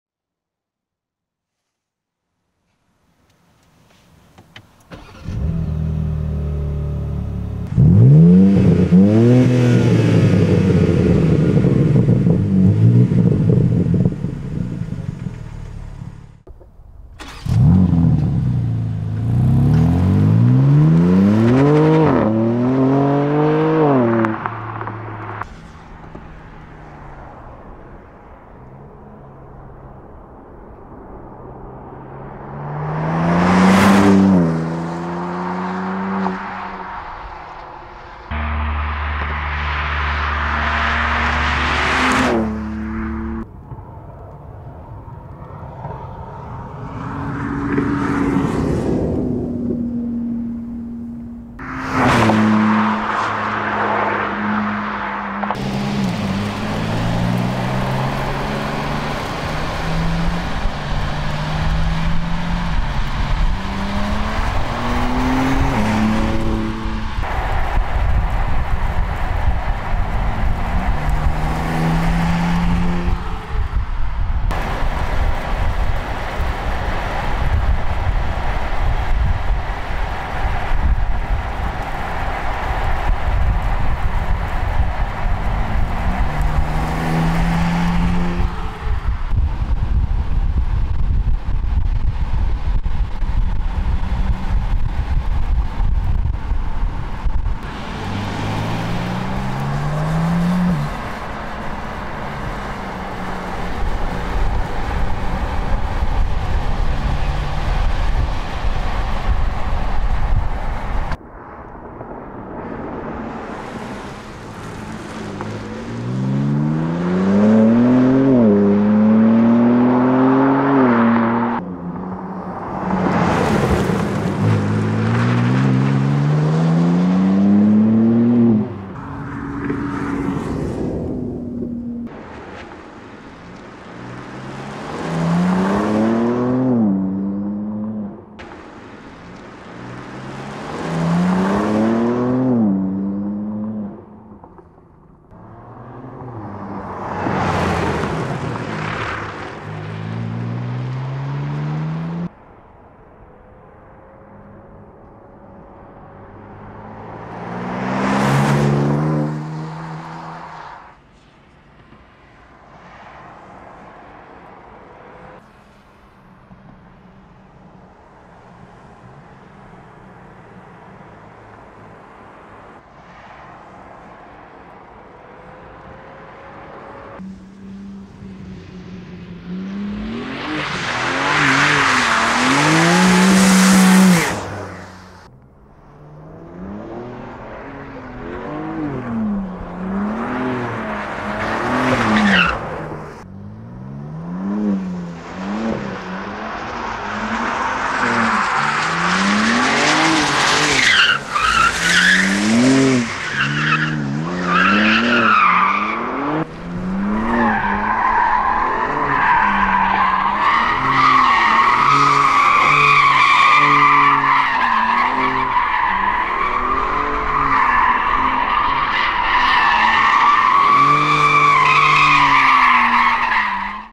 new BMW M4 Competition Coupe ( DRIVING SOUND )
The new BMW M4 Competition Coupé, Portimao Blue

Source:BMW